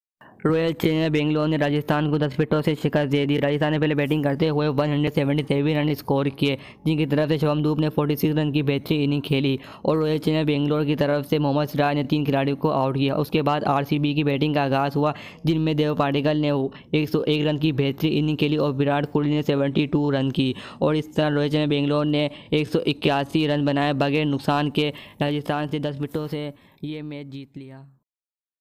Royal Challengers Banglore Win 10 Wickets Against Rajistan Royals Davdutt Padikkal 101 (52)
Devdutt Padikkal Today Innings
Virat Kholi Today Innings vs RR
Rcb vs RR IPL 2021
Today IPL Match
Mohammed Siraj Today Bowling
Rcb win the Match Against RR